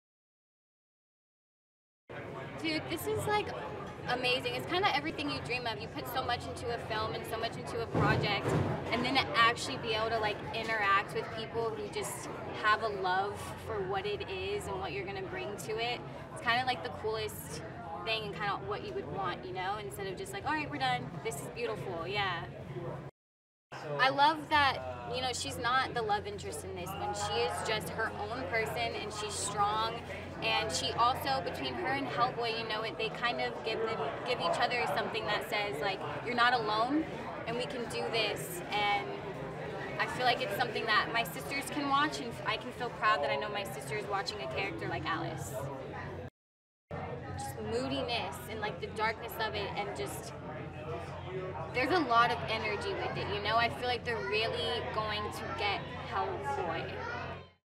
Hellboy NYComiCon 2018 - Itw Sasha Lane (official video)
New-York, Comic Con, October 06 2018

Hellboy is an upcoming American supernatural superhero film based on the Dark Horse Comics character of the same name, distributed by Lionsgate. The film is directed by Neil Marshall and stars David Harbour, Milla Jovovich, Ian McShane, Sasha Lane, Brian Gleeson, Daniel Dae Kim, and Sophie Okonedo. It is a reboot of the Hellboy film series and is the third live-action installment in the film series. The project began as a sequel to Hellboy II: The Golden Army, with Mignola and Andrew Cosby writing the script. Guillermo del Toro declined to return to direct, preferring to direct his own script, and Ron Perlman refused to return without Del Toro's involvement. The project was turned into an R-rated reboot after Marshall was hired as the director. Principal photography began in September 2017 in the United Kingdom and Bulgaria and ended in December 2017. Hellboy is scheduled to be released on April 12, 2019.

Synopsis :
Hellboy prepares to destroy an ancient medieval sorceress who seeks to destroy all of mankind.

Hellboy
Directed by Neil Marshall
Produced by Lawrence Gordon, Lloyd Levin, Mike Richardson
Screenplay by Mike Mignola, Andrew Cosby, Christopher Golden, Aron Coleite
Based on Hellboy by Mike Mignola
Starring David Harbour, Milla Jovovich, Ian McShane, Sasha Lane, Brian Gleeson, Daniel Dae Kim, Sophie Okonedo
Cinematography : Lorenzo Senatore
Edited by Martin Bernfeld
Production companies : Lionsgate, Millennium Films, Campbell Grobman Films, Dark Horse Entertainment, Applebox Entertainment
Distributed by Lionsgate, VVS Films
Release date : April 12, 2019 (United States)

(Source: Wikipedia)